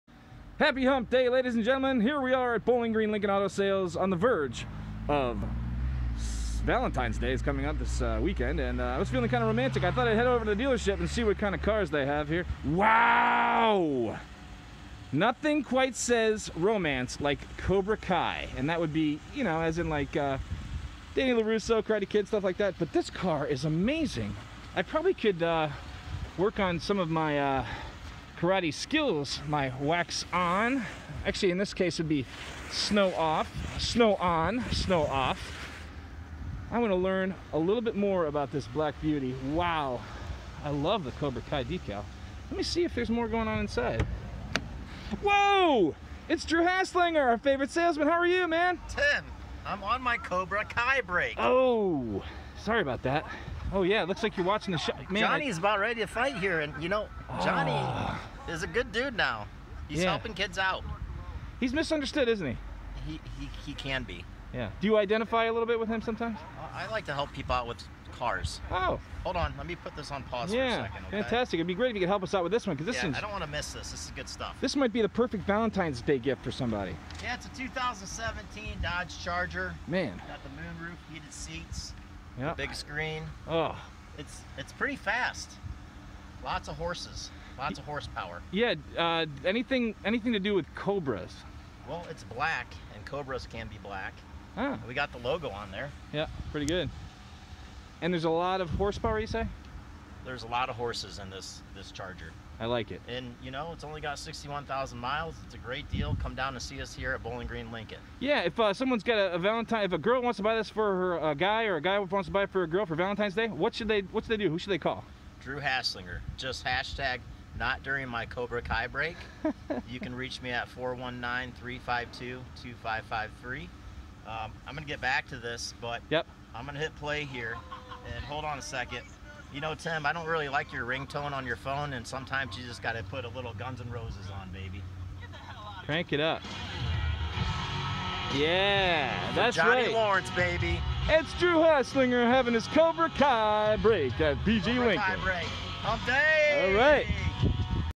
Hump Day Revue 02.10.21 - sponsored by Bowling Green Lincoln Auto Sales
Drew takes a break to show us this beautiful Charger!